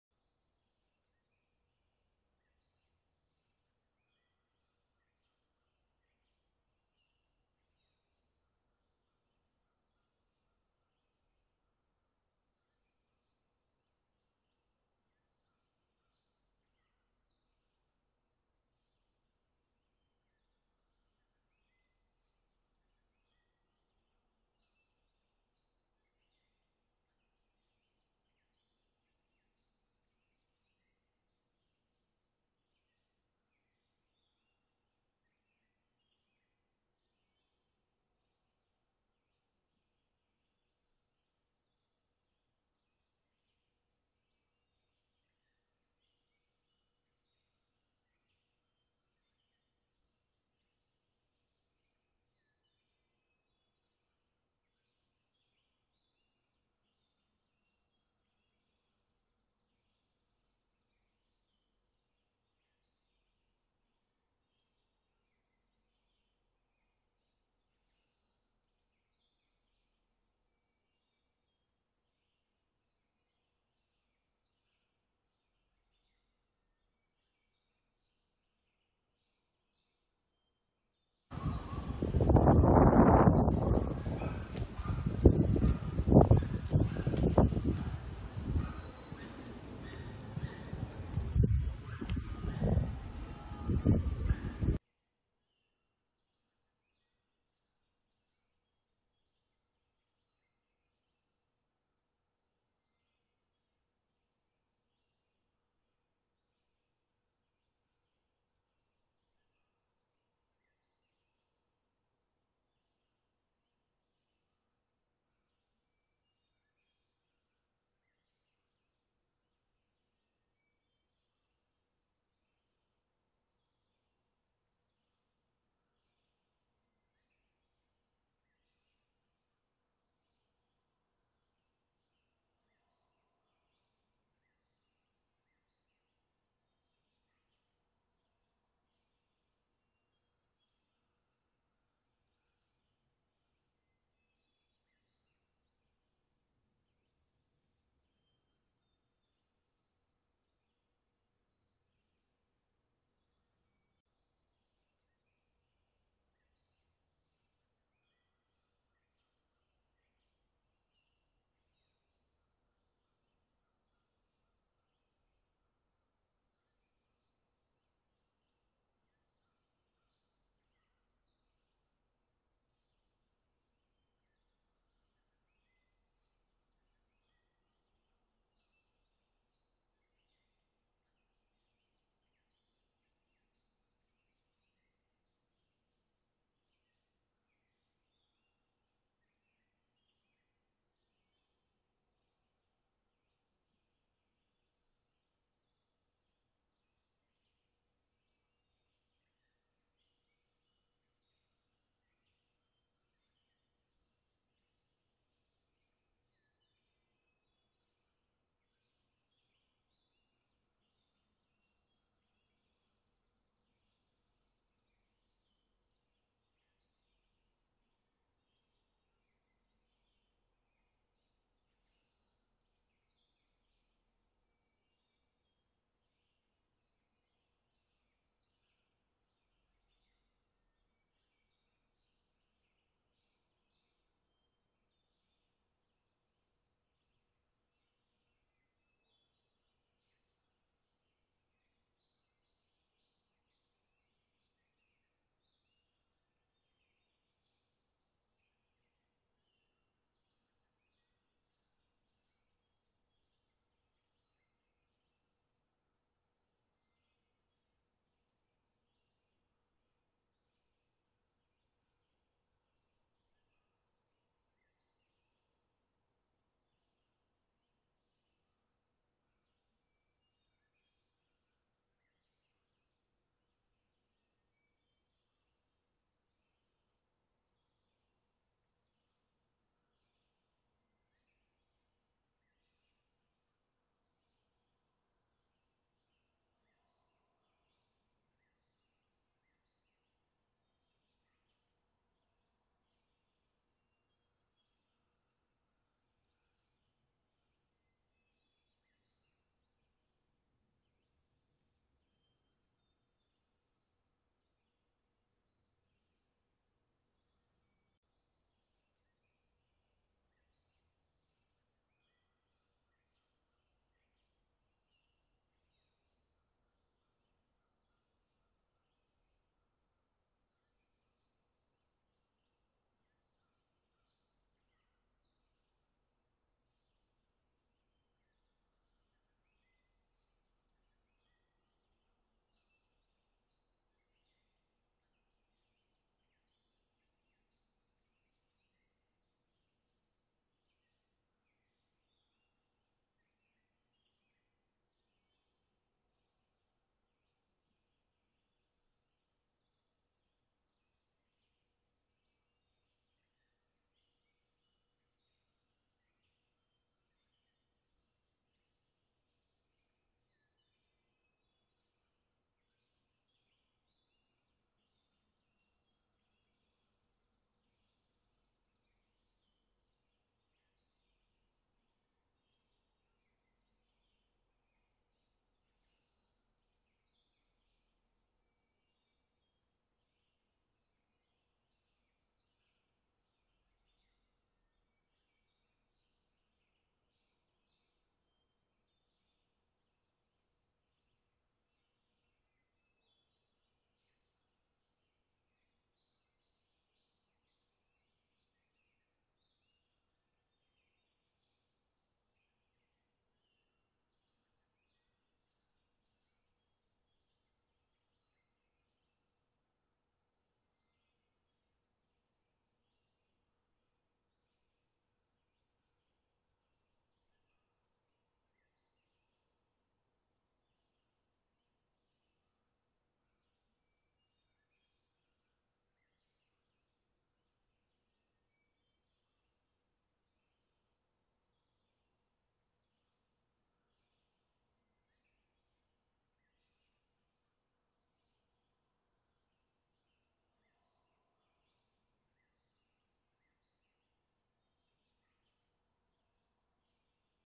Peacocks Opening Feathers & Bird Sound Cockatoo some of the most widely owned birds in the world
The peacock" is commonly used to refer to birds of both sexes. Technically, only male bird is a are peacock.# Cockatoo some of the most widely owned birds in the world | Animal